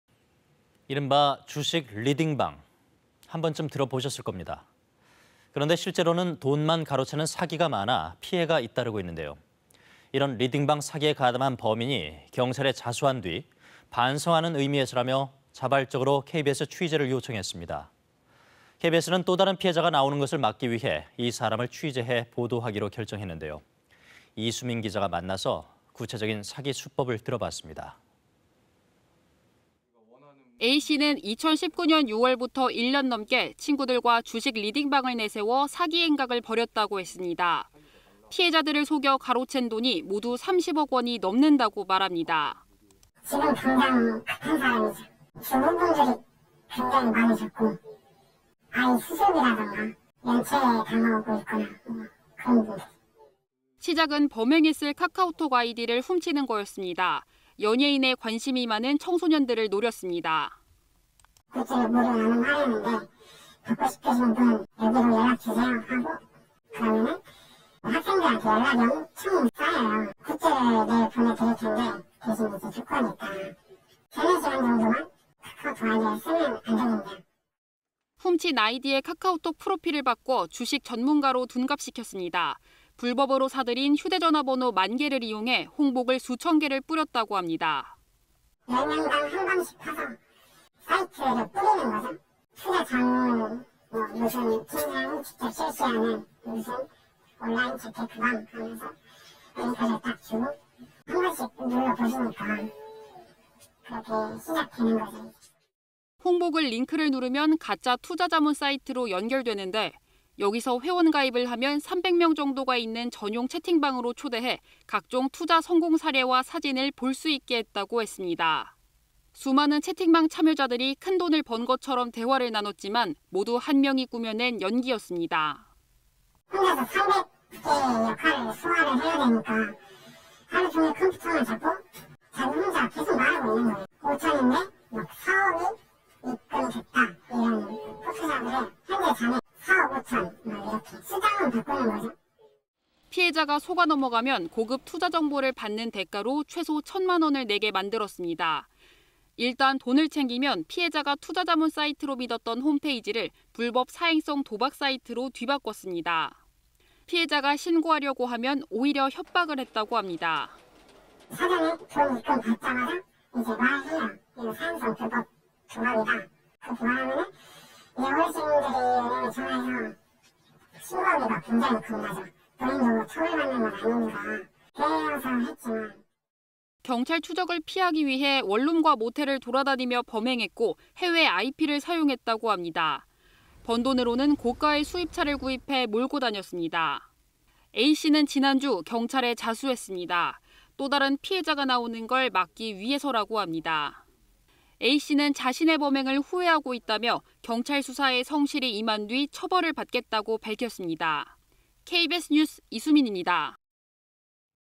“나는 주식 리딩방 사기범이었다”…직접 밝힌 ‘사기 수법’ / KBS 2021.05.26.
이른바 '주식 리딩방' 한번쯤 들어보셨을 겁니다. 그런데 실제론 돈만 가로채는 사기가 많아 피해가 잇따르고 있는데요, 이런 리딩방 사기에 가담한 범인이 경찰에 자수한 뒤, 반성하는 의미에서라며 자발적으로 KBS취재를 요청했습니다.  KBS는 또다른 피해자가 나오는 것을 막기 위해 이 사람을 취재해 보도하기로 결정했는데요.

 [리포트]

A 씨는 2019년 6월부터 1년 넘게  친구들과 주식 리딩방을 내세워 사기 행각을 벌였다고 했습니다.

 피해자들을 속여 가로챈 돈이 모두 30억 원이 넘는다고 말합니다.

 [A 씨/음성변조 : "지금 당장 돈이 급한 사람이죠. 주부분들이 굉장히 많으셨고. 아이 수술비라든가 연체 당한 거 있거나 그런 분들도…."]

 시작은 범행에 쓸 카카오톡 아이디를 훔치는 거였습니다.

 연예인에 관심이 많은 청소년들을 노렸습니다.

  ["(연예인) 굿즈를 무료 나눔하려는데 받고 싶으신 분 뭐 여기로 연락주세요. 그러면 학생들한테 연락이 엄청 쌓여요. 굿즈를 내일 보내드릴테니 대신 조건이 있다. 3, 4시간 정도만 카카오톡 아이디를 쓰면 안 되겠느냐."]

 훔친 아이디의 카카오톡 프로필을 바꿔  주식 전문가로 둔갑시켰습니다. 불법으로 사들인 휴대전화 번호 만 개를 이용해  홍보글 수천 개를 뿌렸다고 합니다.

 ["10명당 한 방씩 (채팅방을) 파서 사이트를 뿌리는 거죠. 투자자문 무슨 팀장이 직접 실시하는 무슨 온라인 재테크방하면서 링크를 딱 주고. 한번씩 눌러보시니까 그렇게 시작되는 거죠."]

 홍보글 링크를 누르면 가짜 투자 자문 사이트로 연결되는데 여기서 회원 가입을 하면 300명 정도가 있는 전용 채팅방으로 초대해 각종 투자 성공 사례와 사진을 볼 수 있게 했다고 했습니다.

 수많은 채팅방 참여자들이 큰돈을 번 것처럼 대화를 나눴지만 모두 한 명이 꾸며낸 연기였습니다.

 [A 씨/음성변조 : "혼자서 300개의 역할을 소화를 해야 되니까. 하루종일 컴퓨터만 잡고 자기 혼자 계속 말하고 있는 거예요. (투자금) 5천인데 막 4억이 입금됐다, 이런 포토샵을 해요. 현재 잔액 4억 5천, 이렇게 숫자만 바꾸는 거죠."]

 피해자가 속아 넘어가면 고급 투자 정보를 받는 대가로 최소 천만 원을 내게 만들었습니다.

 일단 돈을 챙기면 피해자가 투자 자문 사이트로 믿었던 홈페이지를 불법 사행성 도박 사이트로 뒤바꿨습니다.

 피해자가 신고하려고 하면, 오히려 협박을 했다고 합니다.

[A 씨/음성변조 : "사전에 돈 입금 받자마자 말해요 이거 사행성 불법 도박이다. 그렇게 말하면, 어르신들이 은행에 전화해서 신고하기가 굉장히 겁나죠. 본인도 처벌받는 건 아닌가. 꾀어서 했지만…."]

 경찰 추적을 피하기 위해 원룸과 모텔을 돌아다니며 범행했고, 해외 IP를 사용했다고 합니다.

 번 돈으로는 고가의 수입차를 구입해 몰고 다녔습니다.

 A 씨는 지난주 경찰에 자수했습니다.

 또다른 피해자가 나오는 걸 막기 위해서라고 합니

▣ 제보 하기
◇ 카카오톡 : 'KBS제보' 검색